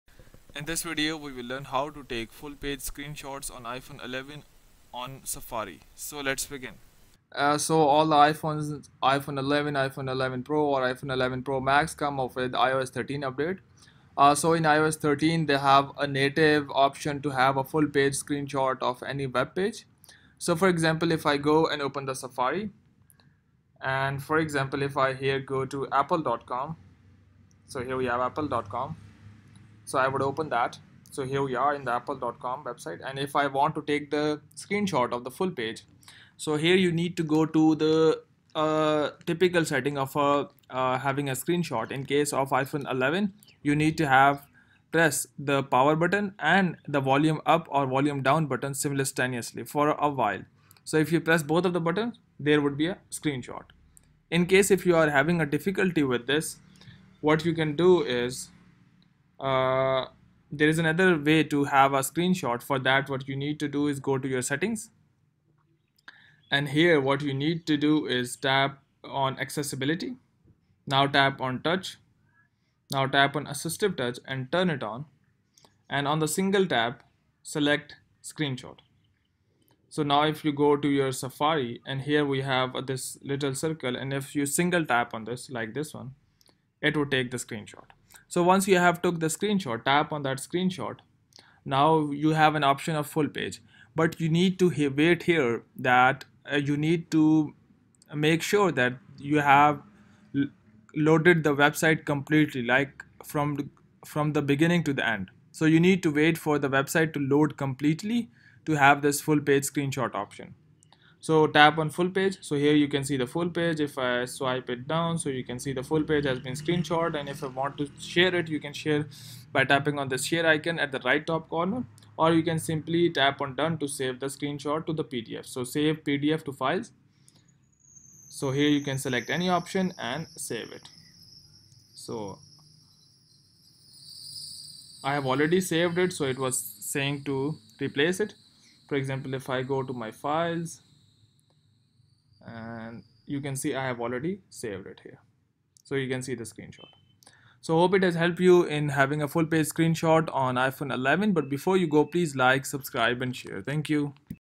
iPhone 11 : How to Take Full Page Screenshot in iPhone 11
Learn How to Take Full Page Screenshot in iPhone 11. It is simple process to take full screen screenshot on iPhone 11, follow this video.

0:00 Intro
0:05 Take Full Page Screenshot in iPhone 11